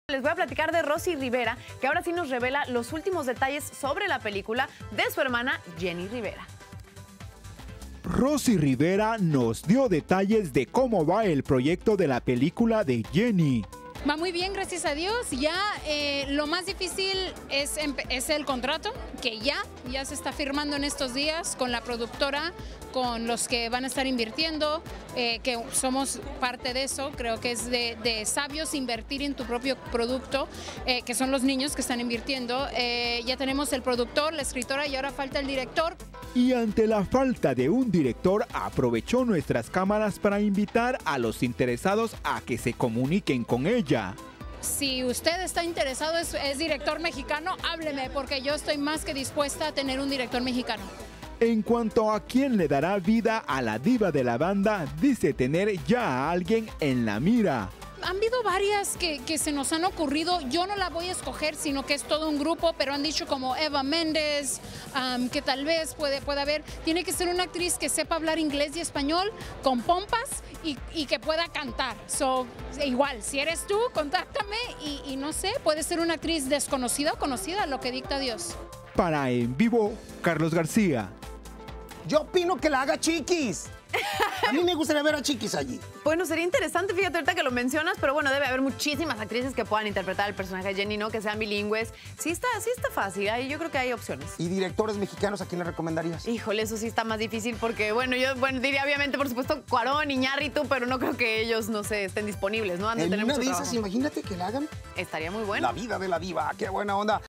Rosie Rivera revela que toda la familia invertirá dinero para la película de Jenni - En Vivo
Rosie Rivera revela que toda la familia invertirá dinero para la película de Jenni